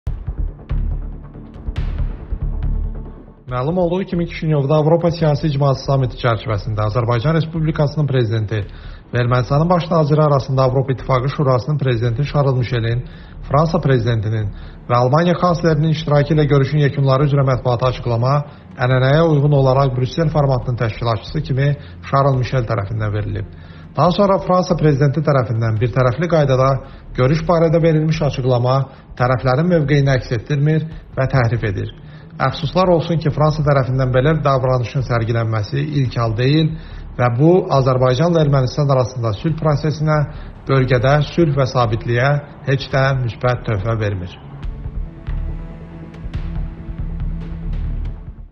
XİN-dən Makronun Kişineu görüşündən sonra verdiyi açıqlamaya cavab
Kişineuda “Avropa Siyasi Birliyi”nin 2-ci Zirvə toplantısı çərçivəsində Azərbaycan Prezidenti İlham Əliyev və Ermənistanın Baş naziri Nikol Paşinyan arasında Aİ Şurasının Prezidenti Şarl Mişelin, Fransa Prezidenti Emmanuel Makronun və Almaniya kansleri Olaf Şoltsun iştirakı ilə görüşün yekunları üzrə mətbuata açıqlama ənənəyə uyğun olaraq Brüssel formatının təşkilatçısı kimi Şarl Mişel tərəfindən verilib.
Bu barədə Xarici İşlər Nazirliyinin mətbuat katibi Ayxan Hacızadə Emmanuel Makron tərəfindən Azərbaycan və Ermənistan liderləri arasında Kişineu görüşünün nəticələrinə dair yayılmış bəyanat barədə yerli mətbuatın sualına cavabında bildirib.
“Fransa Prezidenti tərəfindən birtərəfli qaydada görüş barədə verilmiş açıqlama tərəflərin mövqeyini əks etdirmir və təhrif edir. Əfsuslar olsun ki, Fransa tərəfindən belə davranışın sərgilənməsi ilk hal deyil və bu, Azərbaycanla Ermənistan arasında sülh prosesinə, bölgədə sülh və sabitliyə heç də müsbət töhfə vermir”, – deyə XİN rəsmisi qeyd edib.